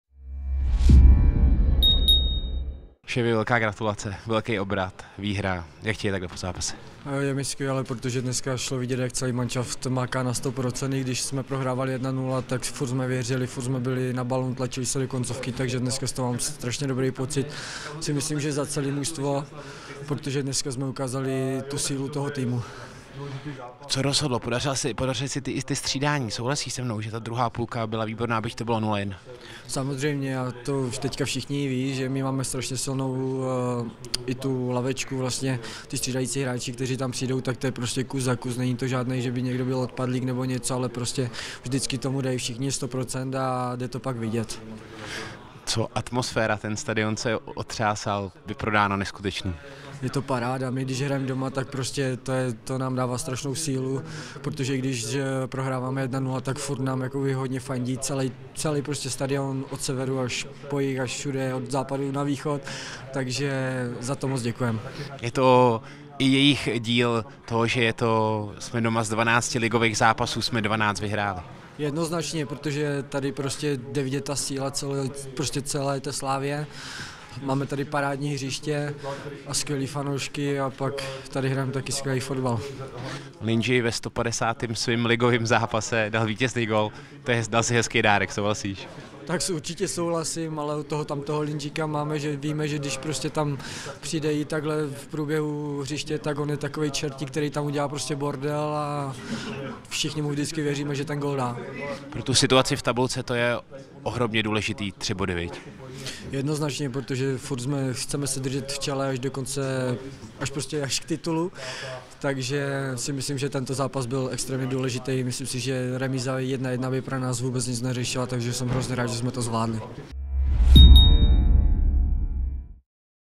OHLASY | Petr Ševčík po výhře nad Plzní
Petr Ševčík po obratu proti Plzni (2:1) v pozápasovém rozhovoru vyzdvihl fanoušky, přínos střídajících hráčů v čele s Ondřejem Lingrem a vzpomenul také letošní ligovou domácí neporazitelnost.